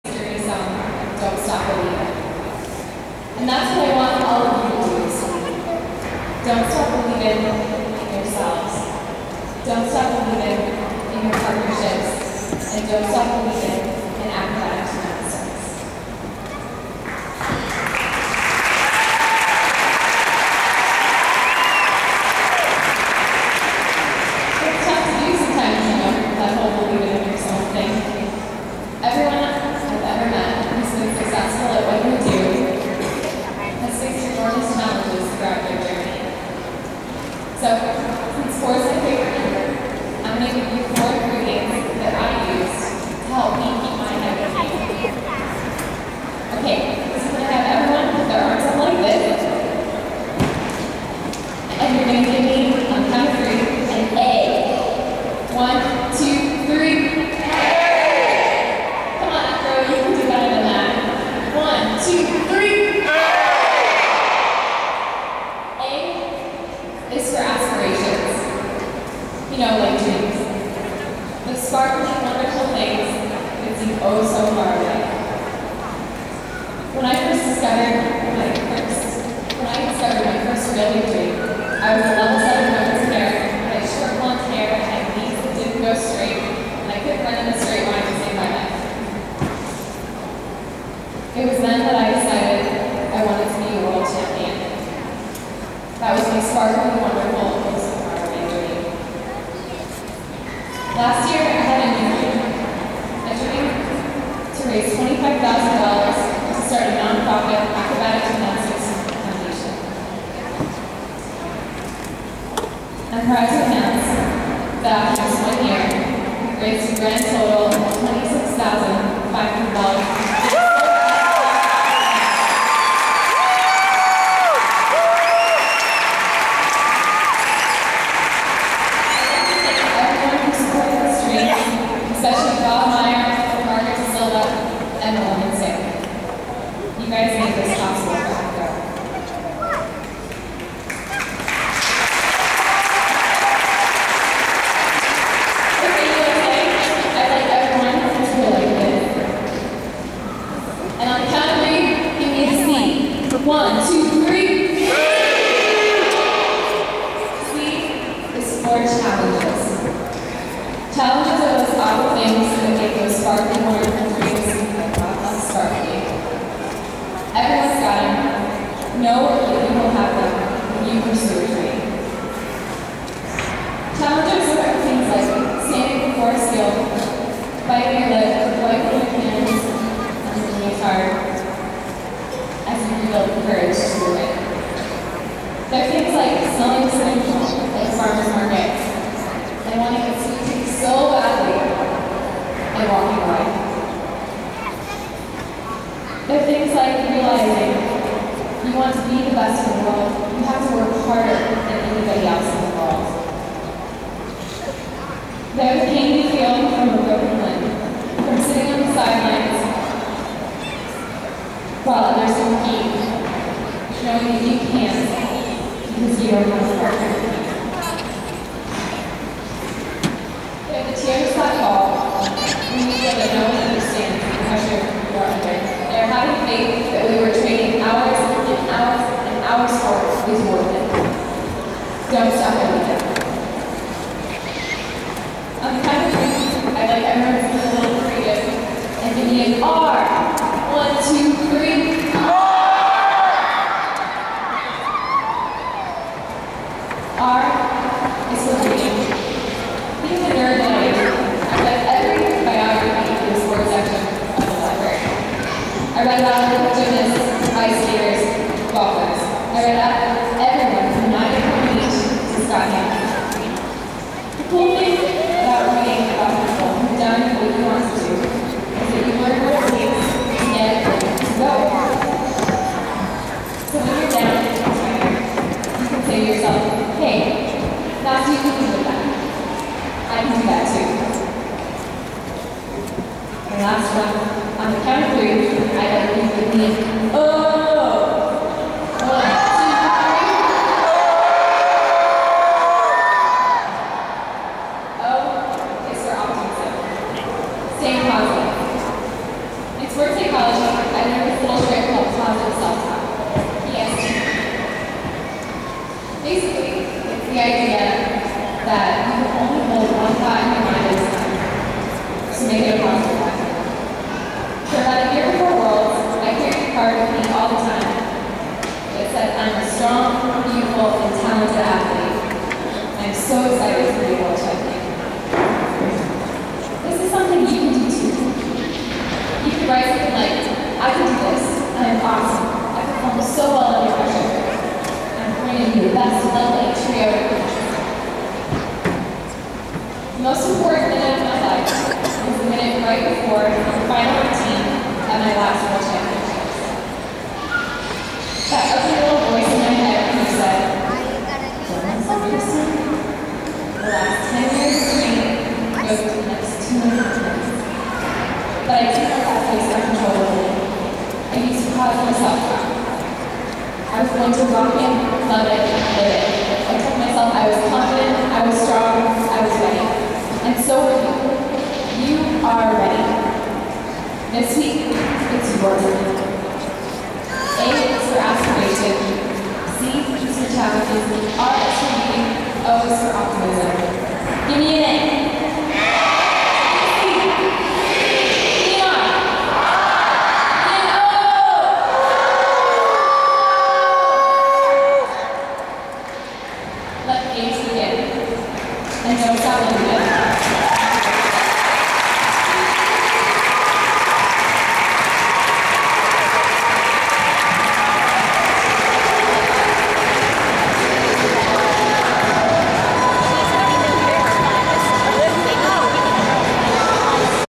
Kristin Allen Acro Nationals 2012
World Champion Kristin Allen speaks at the opening ceremony of the  Acrobatic Gymnastics National Championships in Detroit, MI in July 2012.